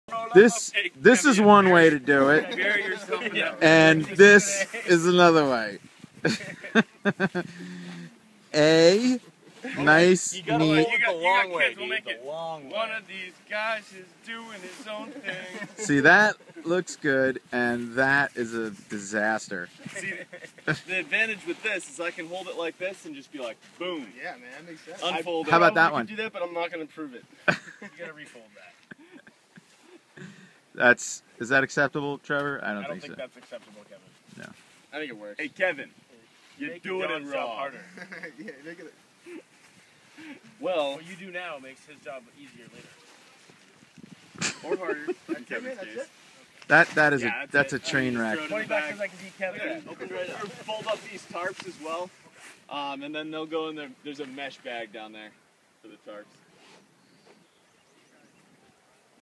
One of these guys is doing his own thing.
Video Aug 09, 12 17 32 PM - packing up camp on Day 2 am.  Salmon River, ID